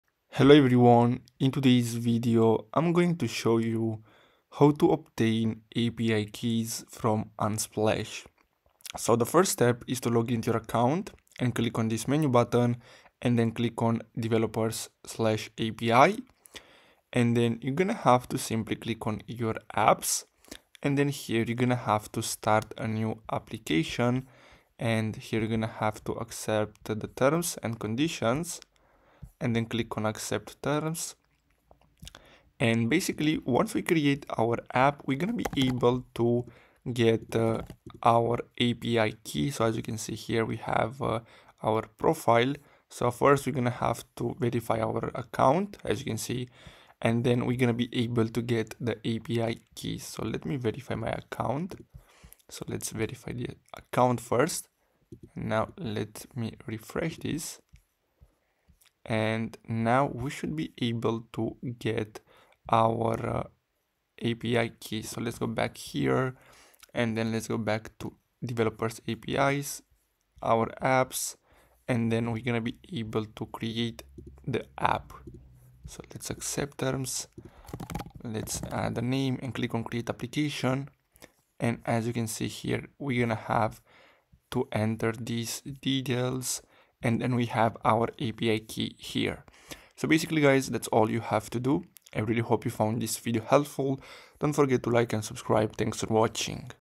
How to Obtain API Keys from Unsplash (2024)
Learn how to easily obtain API keys from Unsplash in this step-by-step tutorial. We'll guide you through the process, from creating an Unsplash account to generating your unique API key. Perfect for developers and photographers looking to integrate Unsplash's vast image library into their applications.

Thanks for visiting Tech Nexus – where tech tutorials meet innovation!